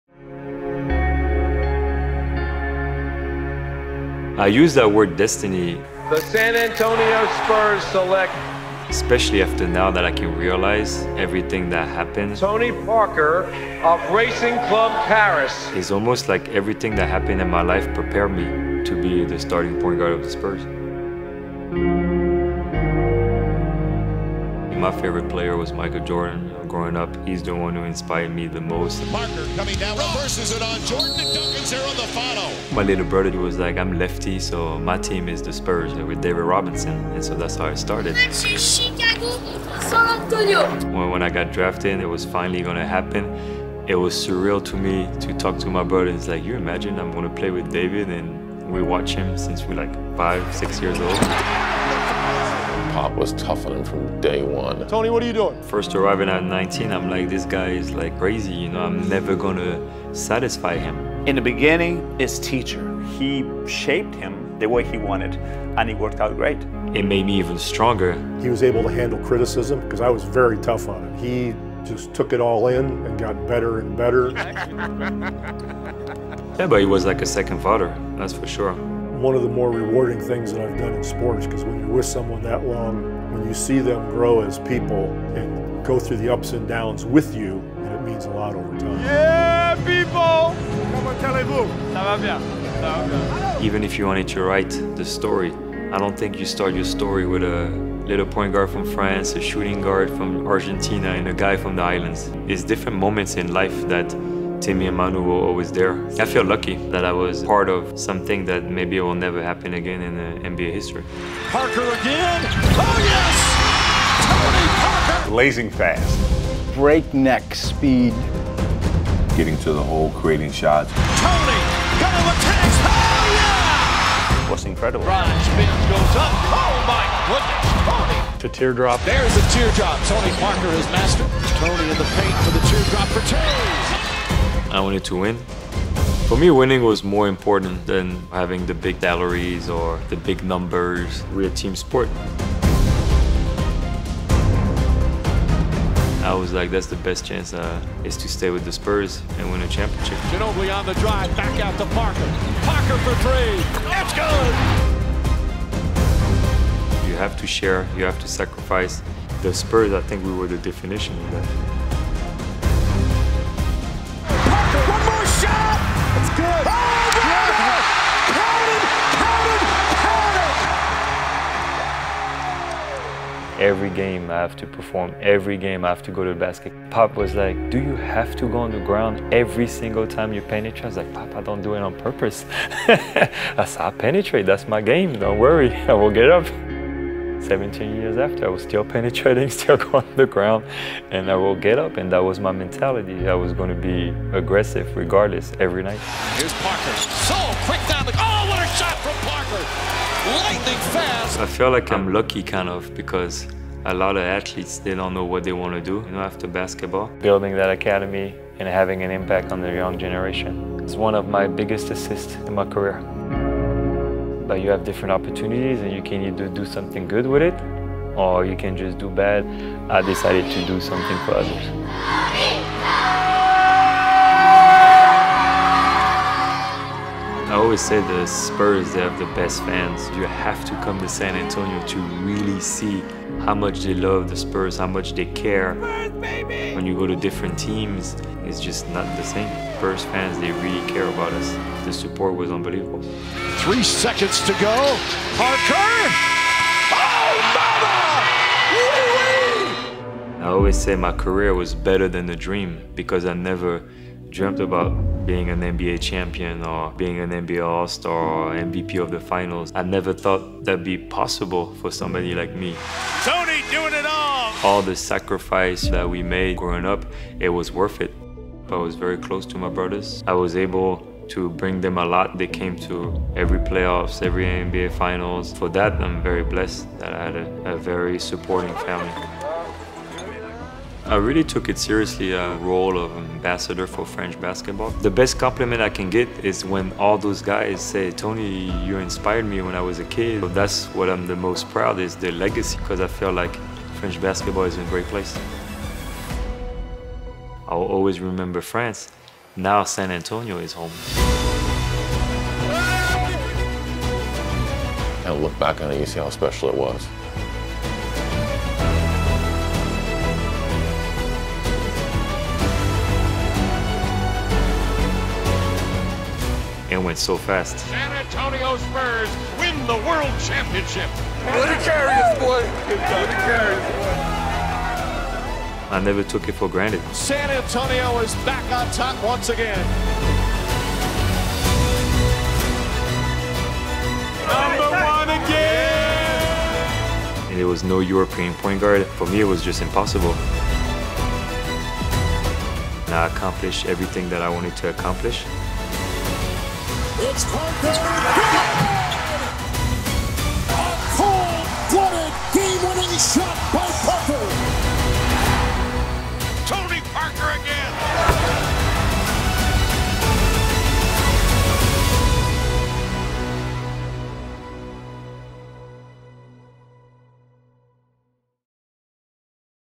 A Look Back at the Career of Tony Parker
Tribute video for Tony Parker's Jersey Retirement Ceremony.